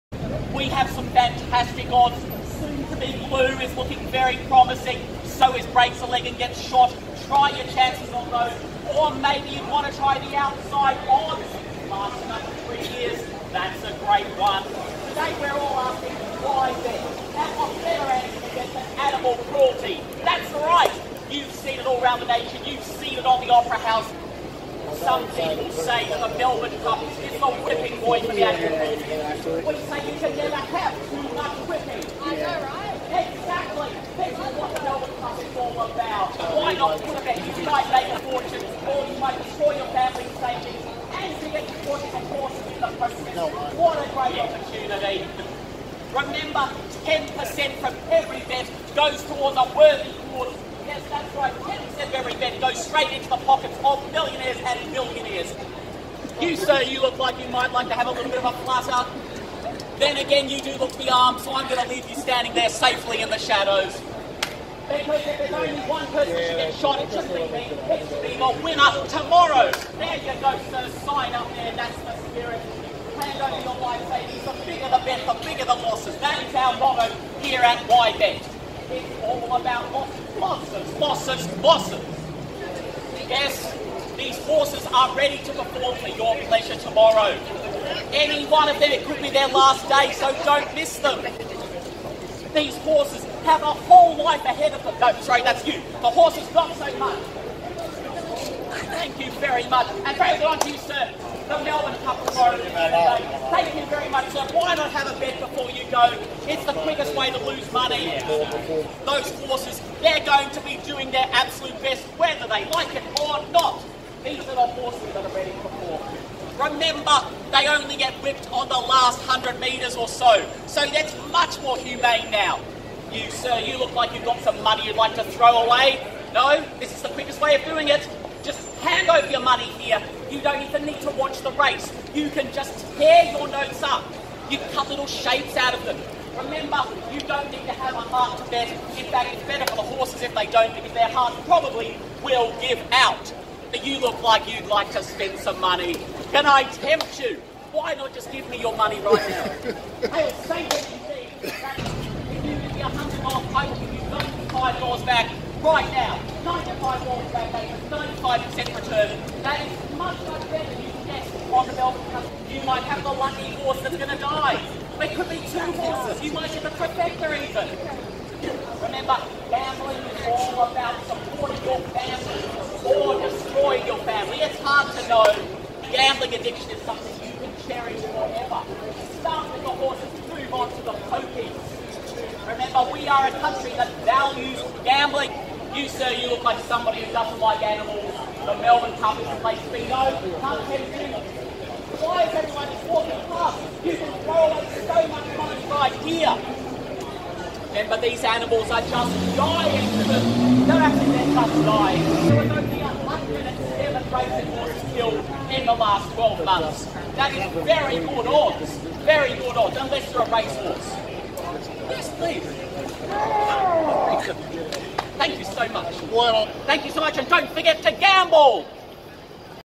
Honest horse racing spruiking for the Melbourne Cup
What would happen if I promoted the *real* outcomes of horse racing - but at my local betting van?

My city supports the struggling gambling industry by installing "pop up" betting vans all around town for the Melbourne Cup - helping people get into gambling *and* the excitement of whipping horses for pleasure.

So I thought I'd go along and support them in return by doing some enthusiastic spruiking...